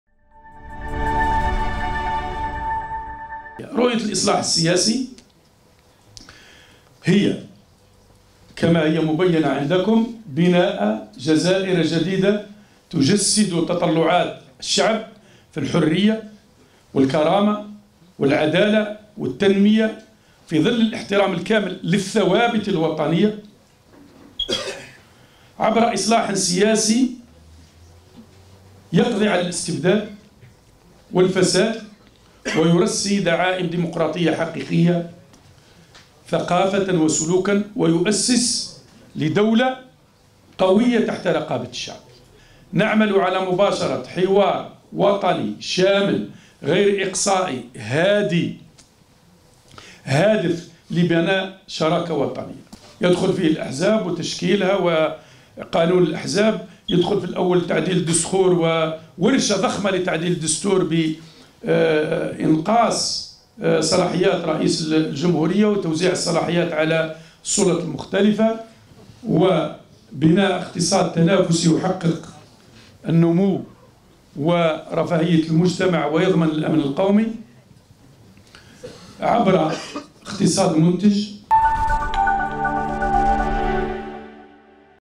عبد القادر بن قرينة يعرض محاور برنامجه الانتخابي
الجزائر (وأج ) - كشف مرشح حزب حركة البناء الوطني  عن أهم محاور برنامجه الانتخابي تحسبا لرئاسيات 12 ديسمبر المقبل، القائم على رؤيته للإصلاح السياسي الذي يهدف لتجسيد تطلعات الشعب في الحرية و الكرامة